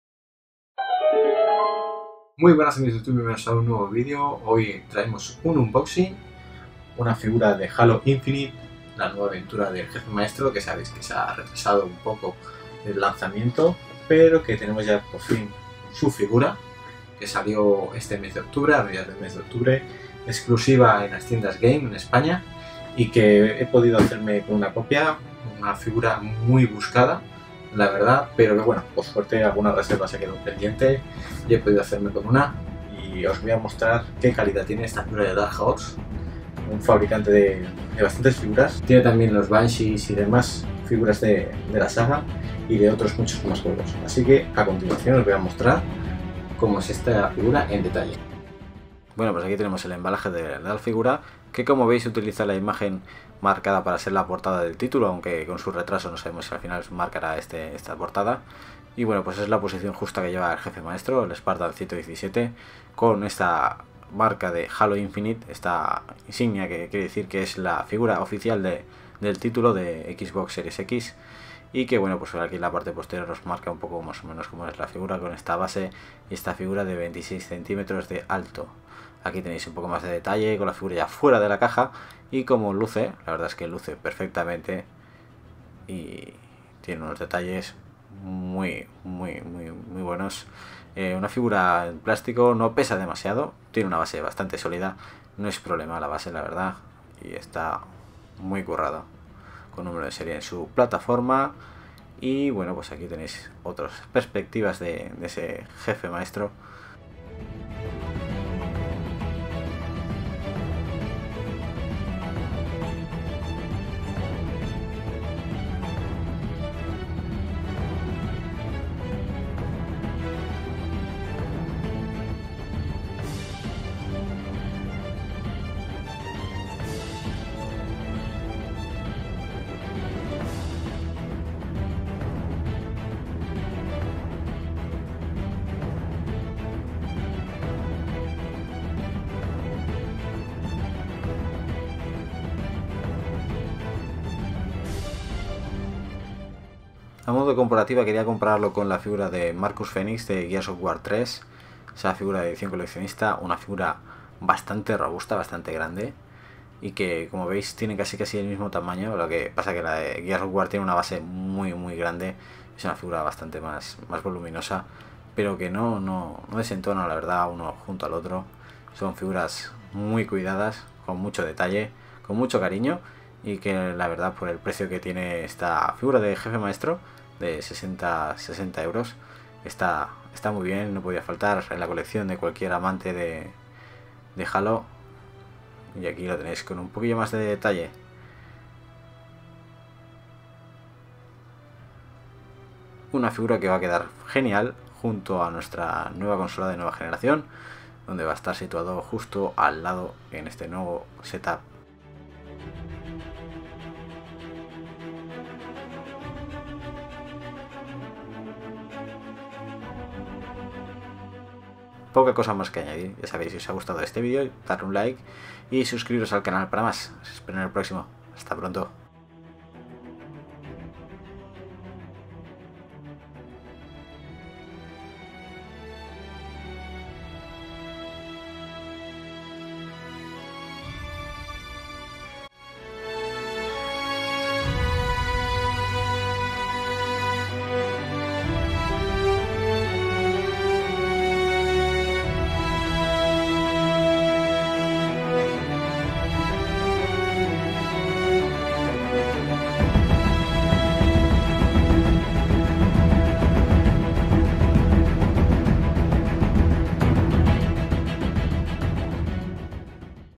Unboxing MASTER CHIEF | HALO INFINITE
Muy buenas, llevo unas semanas con la figura en casa, pero quería compartir con vosotros como es la figura del Jefe Maestro que ha salido a la venta por parte de Dark Horse, y en exclusiva en España en tiendas GAME. Una figura muy cuidada que no puede faltaros en vuestra habitación si soy amantes de la saga.